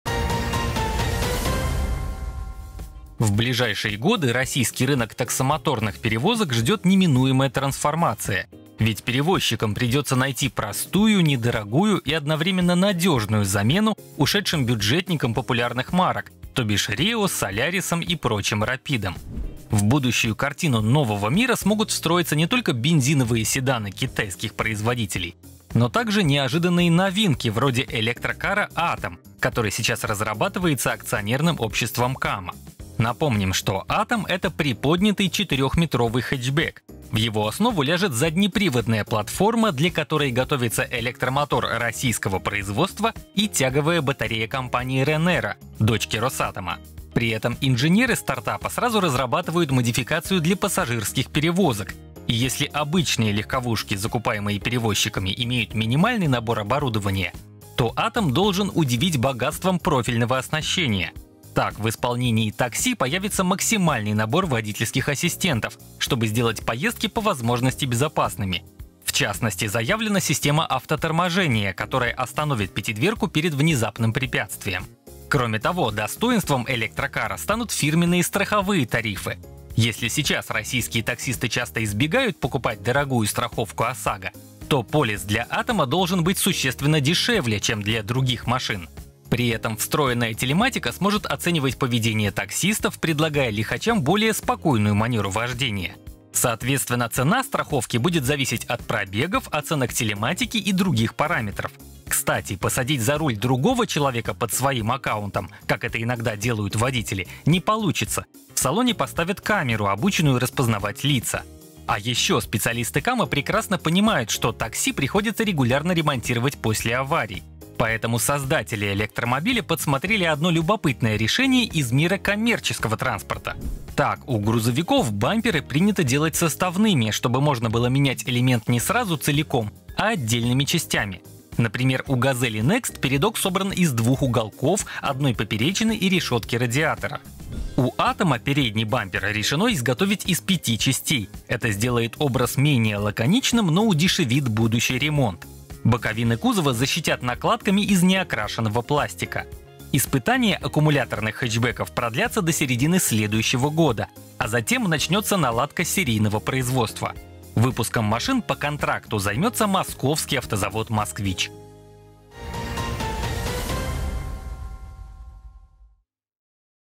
Атом займётся пассажирскими перевозками | Новости с колёс №2740
В ближайшие годы российский рынок таксомоторных перевозок ждёт неминуемая трансформация. Ведь перевозчикам придётся найти простую, недорогую и одновременно надёжную замену ушедшим «бюджетникам» популярных марок, то бишь Рио, Солярисам и прочим Рапидам. В будущую картину нового мира смогут встроиться не только бензиновые седаны китайских производителей, но также неожиданные новинки вроде электрокара Атом, который сейчас разрабатывается акционерным обществом «Кама». Напомним, что Атом — это приподнятый четырёхметровый хэтчбек. В его основу ляжет заднеприводная платформа, для которой готовятся электромотор российского производства и тяговая батарея компании «РЭНЕРА» — дочки «Росатома». При этом инженеры стартапа сразу разрабатывают модификацию для пассажирских перевозок. И если обычные легковушки, закупаемые перевозчиками, имеют минимальный набор оборудования, то Атом должен удивить богатством профильного оснащения. Так, в исполнении Такси появится максимальный набор водительских ассистентов, чтобы сделать поездки по возможности безопасными. В частности, заявлена система автоторможения, которая остановит пятидверку перед внезапным препятствием. Кроме того, достоинством электрокара станут фирменные страховые тарифы. Если сейчас российские таксисты часто избегают покупать дорогую страховку ОСАГО, то полис для Атома должен быть существенно дешевле, чем для других машин. При этом встроенная телематика сможет оценивать поведение таксистов, предлагая лихачам более спокойную манеру вождения. Соответственно, цена страховки будет зависеть от пробегов, оценок телематики и других параметров. Кстати, посадить за руль другого человека под своим аккаунтом, как это иногда делают водители, не получится! В салоне поставят камеру, обученную распознавать лица. А ещё специалисты Камы прекрасно понимают, что такси приходится регулярно ремонтировать после аварий. Поэтому создатели электромобиля подсмотрели одно любопытное решение из мира коммерческого транспорта. Так, у грузовиков бамперы принято делать составными, чтобы можно было менять элемент не сразу целиком, а отдельными частями. Например, у ГАЗели Next передок собран из двух уголков, одной поперечины и решётки радиатора. У Атома передний бампер решено изготовить из пяти частей — это сделает образ менее лаконичным, но удешевит будущий ремонт. Боковины кузова защитят накладками из неокрашенного пластика. Испытания аккумуляторных хэтчбеков продлятся до середины следующего года, а затем начнётся наладка серийного производства. Выпуском машин по контракту займётся Московский автозавод «Москвич».